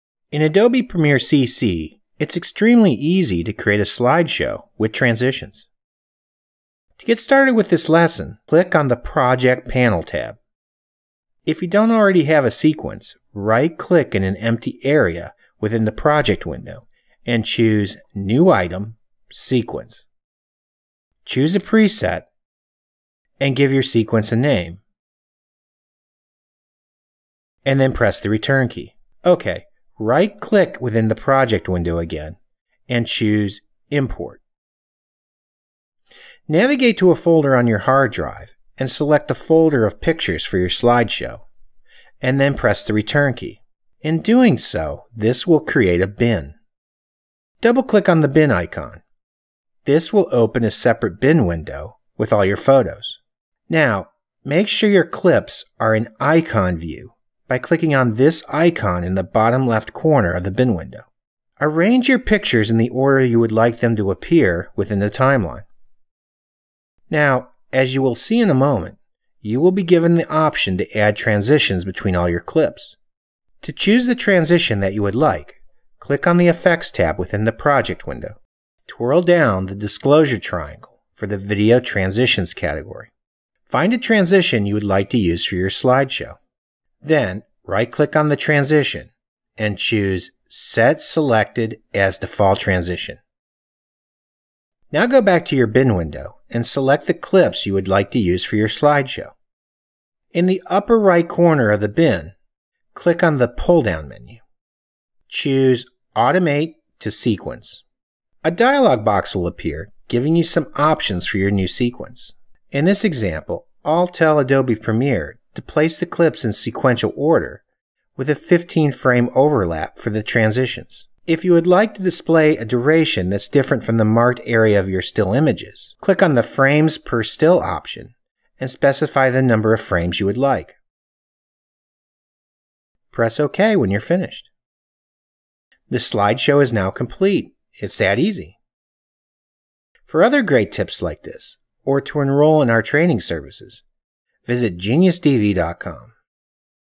Automated Slideshow in Adobe Premiere CC
Learn how to create an automated slideshow with transitions using Adobe Premiere